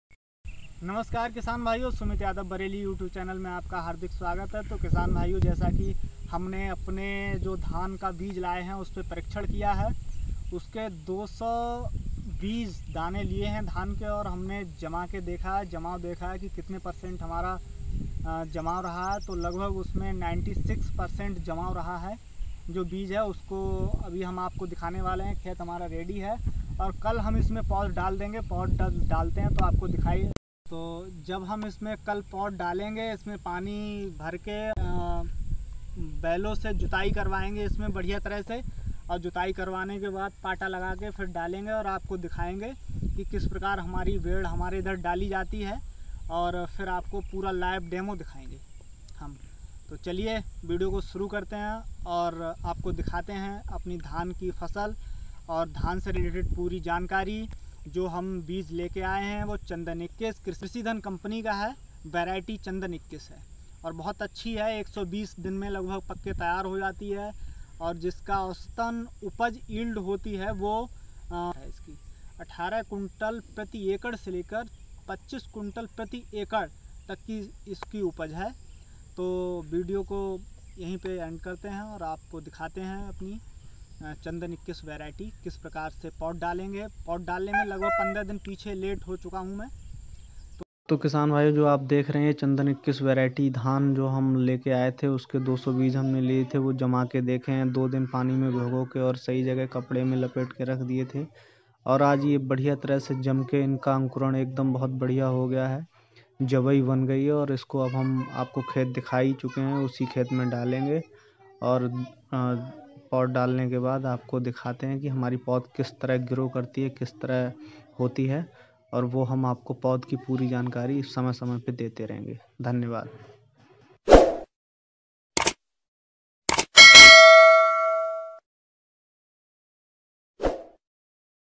Chandan 21| चंदन21 कमसमय हर प्रकारकी मिट्टीमें उपयोगी अच्छी पैदावार 120 दिनमें ल. तैयार अच्छी बिक्री
namaskar Kisan bhaiyon Sumit Yadav Bareilly YouTube channel Mein aapka Hardik Swagat Hai To Kisan bhaiyon Aaj Ham do Jankari aapko dene Ja Rahe Hain vah Jankari Hai dhaan Chandan Chandan 21 hybrid Dhan hai 120 Din Mein pakka taiyar ho jata hai lagbhag Har Prakar Ki Mitti Mein upjau Adhik paidavar acchi acchi income bikri ke liye bahut Achcha hai khane mein bhi achcha hai hi Saraswati vahan sviti ke lagbhag jaisa lagta hai khane mein aur bahut hi kam normal lagat mein taiyar ho jata hai koi rok Bhi Nahin Lagta Hai aur pad bhi iski Sasti alag Bhag 200 se 300 rupaye Prati kilo aapko bhi Jiska Mil Jata Hai To aap usko Jarur Laga kar Dekhen Ham usko 3 Sal se Lagate Aaye Har Bar Achcha result Mila Hai achi paidawar Mili Hai khane ke liye Achcha hai sab kam Mein Achcha hai isliye usko Laga Rahe Hain thano preshit Koi Bhi Jankari ho to please Aap Hamare liye comment box Mein Apna Naam number pata bhejiye Ham Aapke Puri madad Har sambhog karne ke liye taiyar hai dhanyvad tu Bane Diye video ke last Tak dikhte Hain aapko ful Jankari Chandan hybrid dhanju krishidhan company ka hai dhanyvad